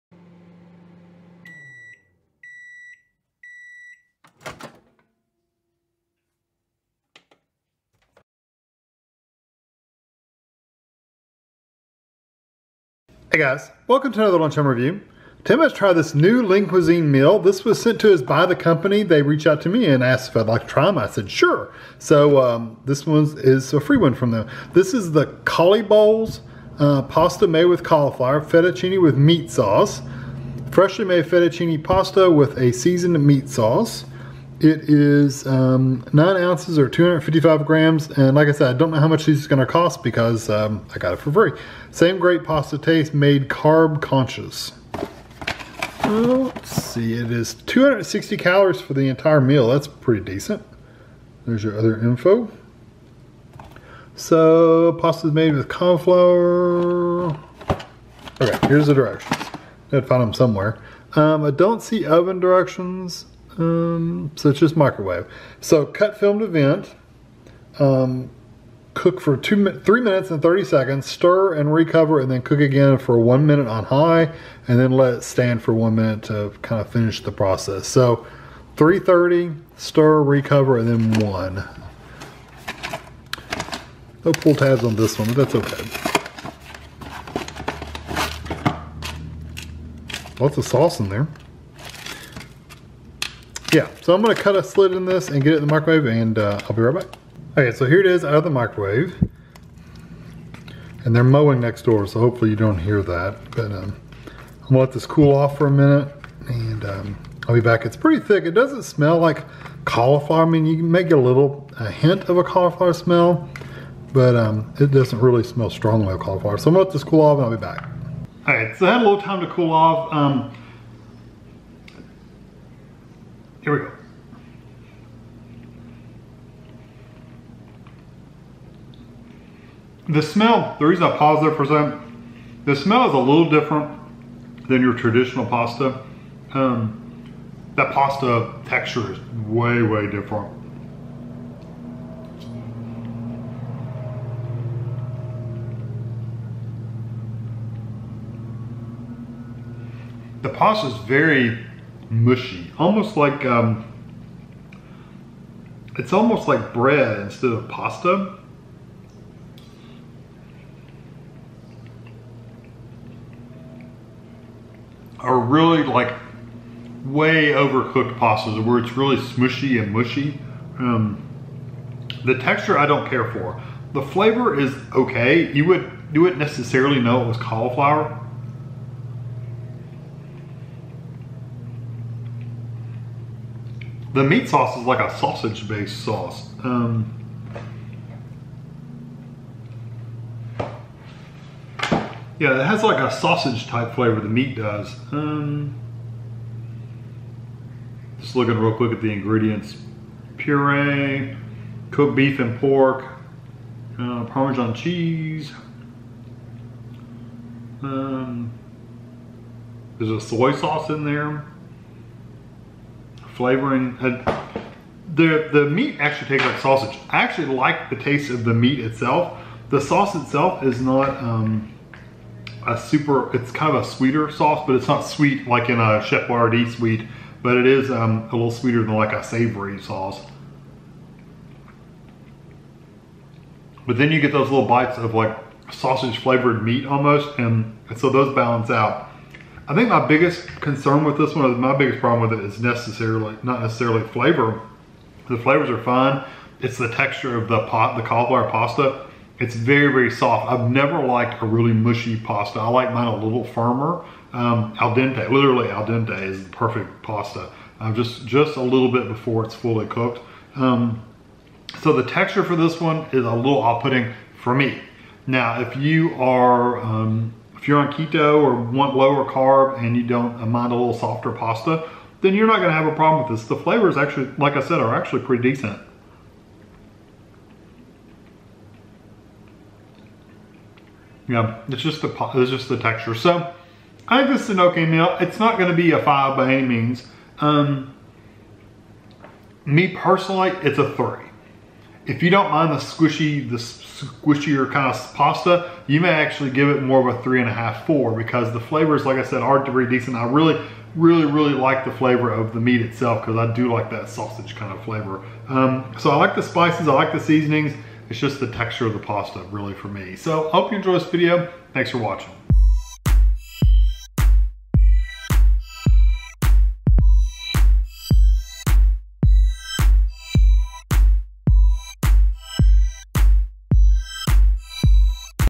Lean Cuisine Cauli’ Bowls Pasta Made with Cauliflower: Fettuccini with Meat Sauce Review
This is a taste test/review of the new Lean Cuisine Cauli’ Bowls Pasta Made with Cauliflower Fettuccini with Meat Sauce meal. It is freshly made Fettuccine pasta with a seasoned meat sauce. It was mailed to me free from Lean Cuisine. 1 Package (255g) = 260 calories